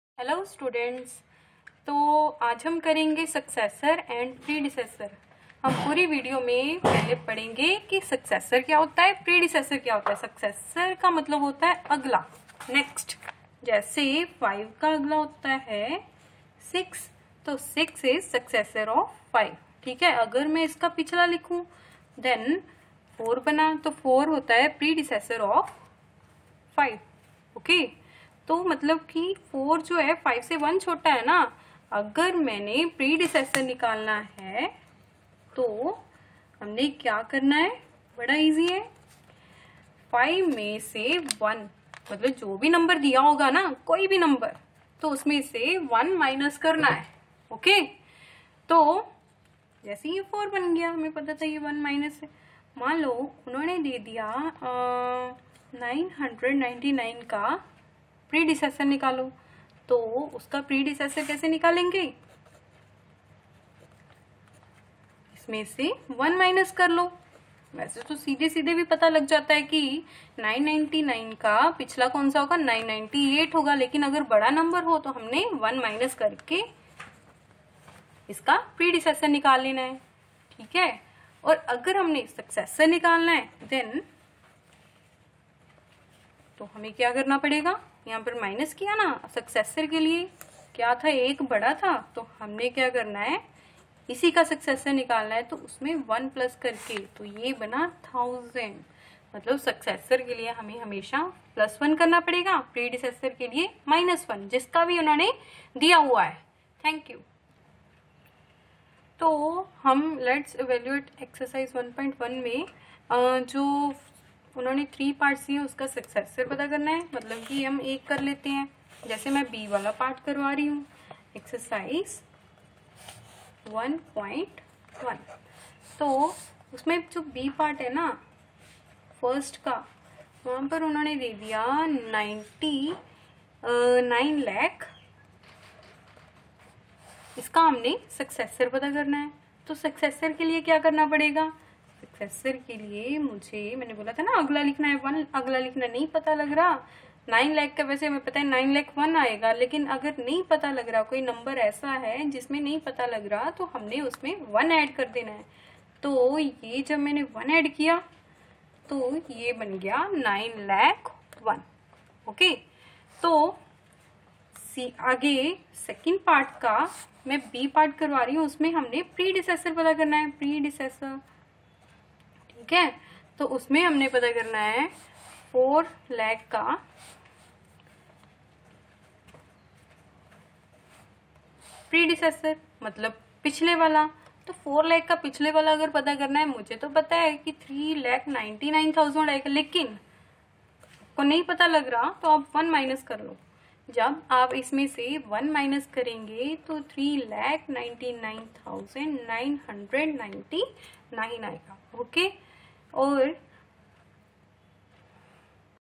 Successor and Predecessor in hindi
Learn to find successor and predecessor in very simple way.
By Ekta